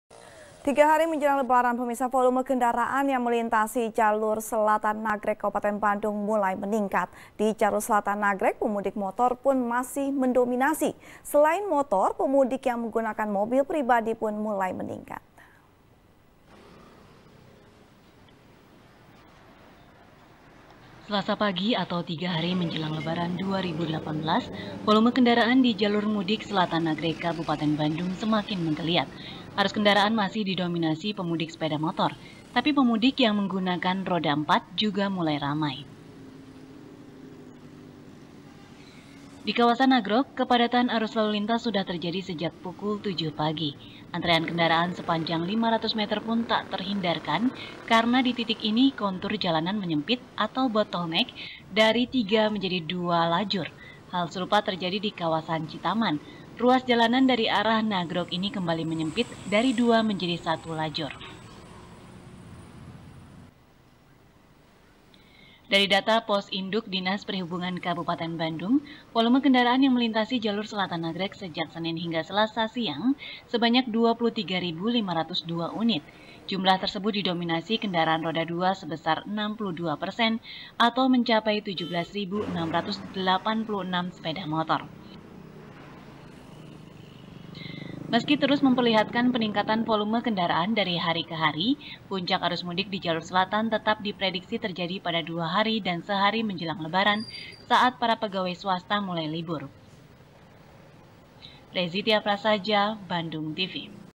Jalur Selatan Nagreg Mulai Padat | SBR | BANDUNG TV
SBR | BANDUNG TV | FARHAT MUMTAZ | Tiga hari menjelang lebaran, volume kendaraan yang melintasi jalur selatan nagreg, kabupaten bandung, mulai meningkat. di jalur seltan nagreg, pemudik motor masih mendominasi. selain motor, pemudik yang mengunakan mobil pribadi pun mulai meningkat.